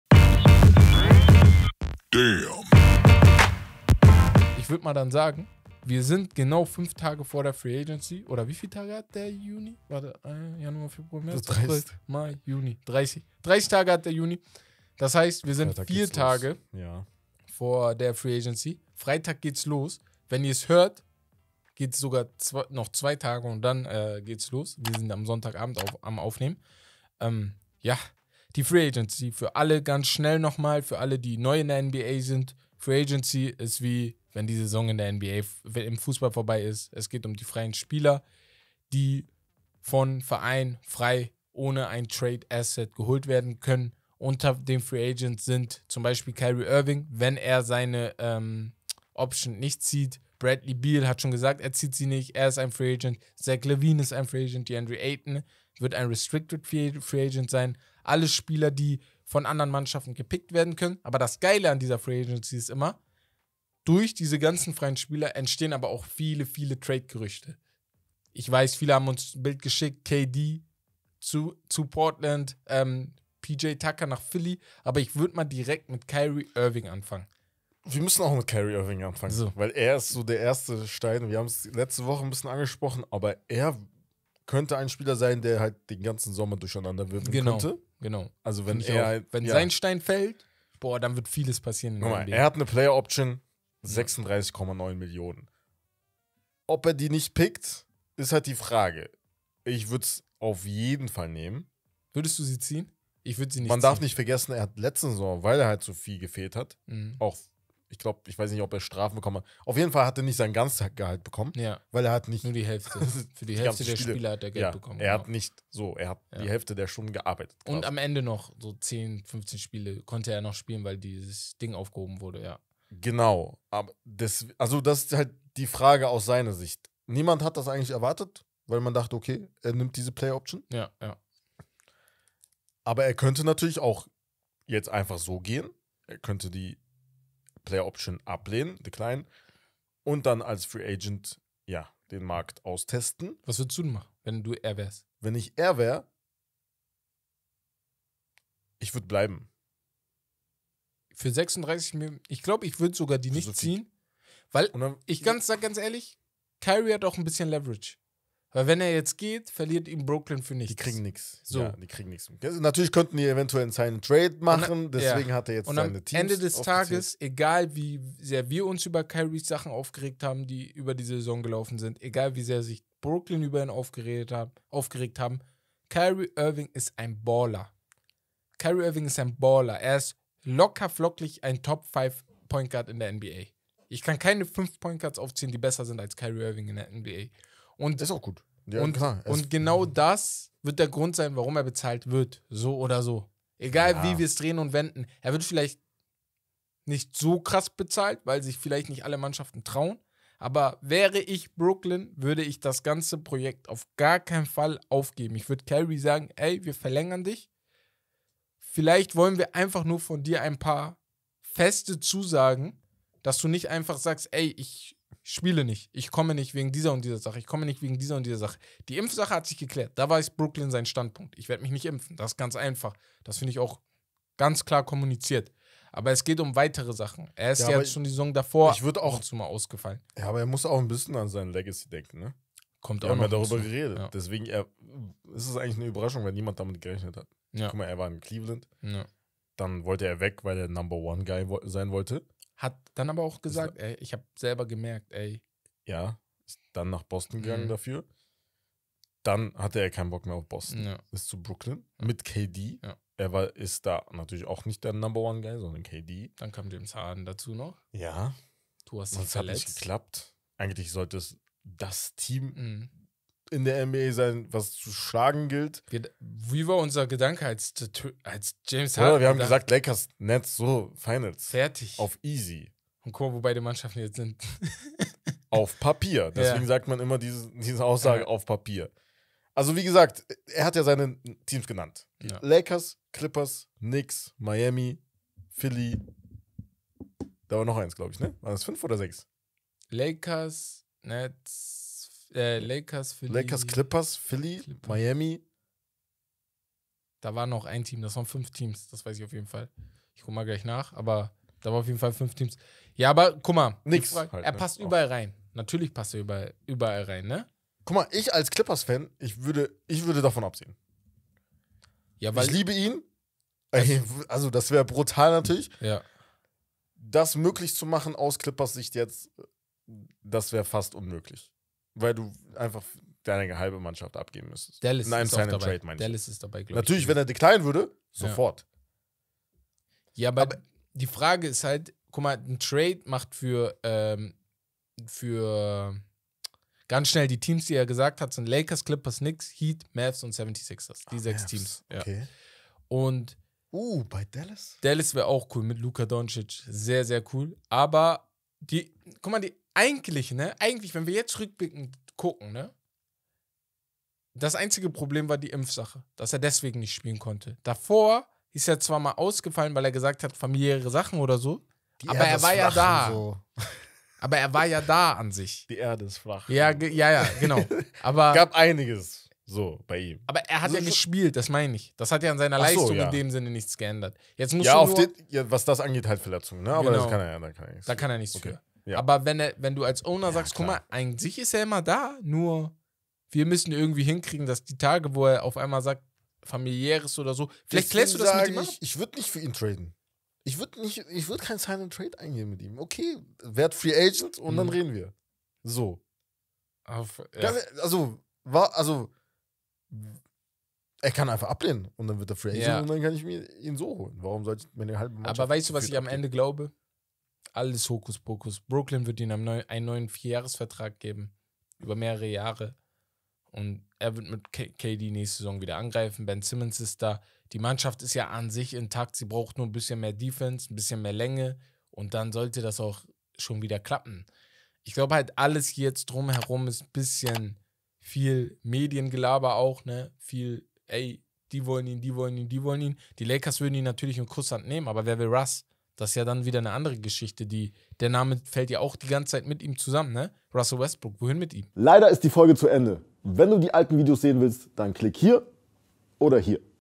Kyrie Irving bleibt am ENDE doch bei den NETS | S&L Podcast
Die Free Agency steht kurz bevor, der Draft ist zu Ende. Wes und Herb haben sich deshalb zusammengesetzt und über einige Spieler im Podcast gesprochen. In diesem Ausschnitt des Podcast geht es um Kyrie und das Theater um ihn herum.

Wir bedanken uns bei der Filmproduktion Manuel Rueda und dem ganzen Team!